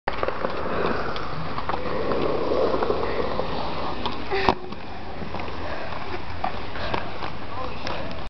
Ashley doing the step up at poco
It was like raining so hard so we had to take this reall fast